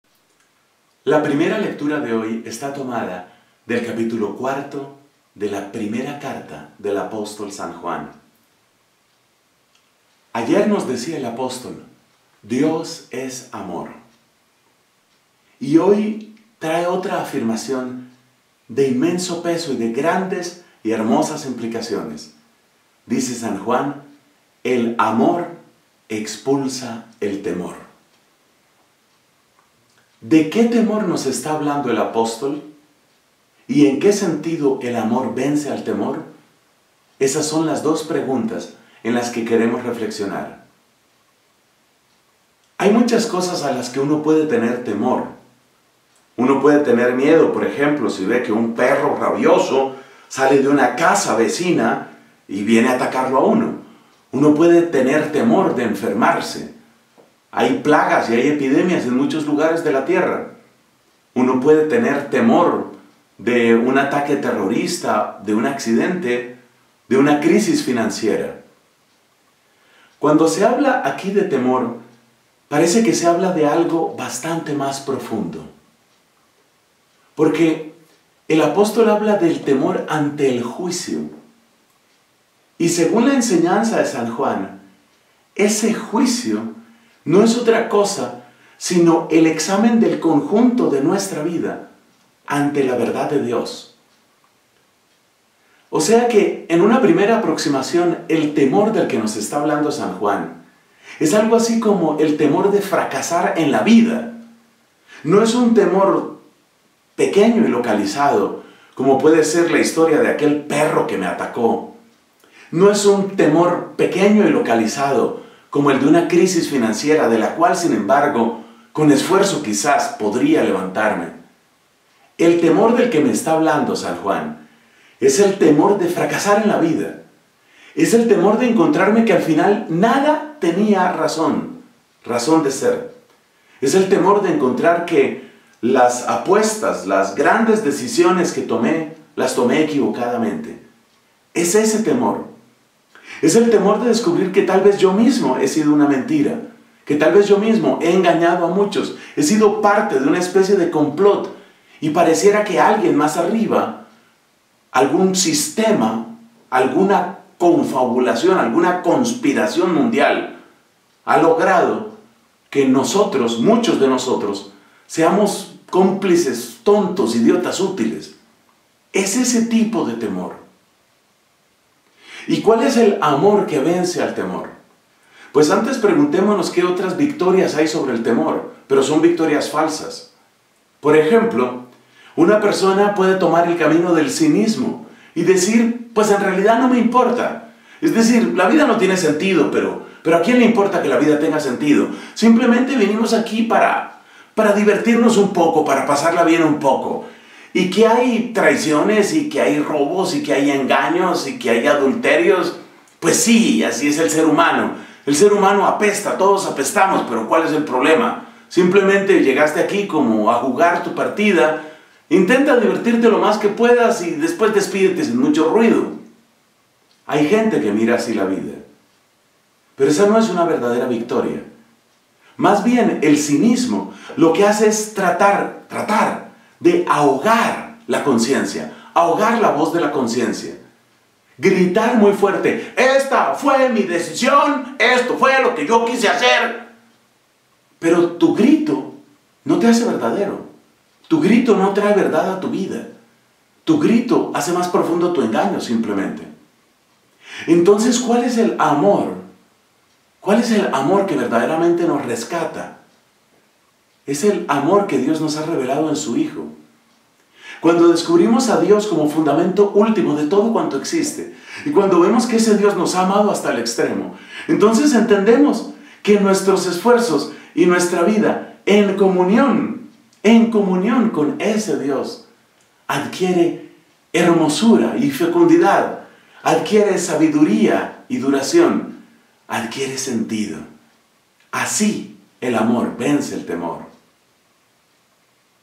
LA GRACIA del Miércoles 7 de Enero de 2015
¿De qué clase de temor nos habla San Juan y de qué clase de amor, cuando nos enseña que el amor expulsa el temor?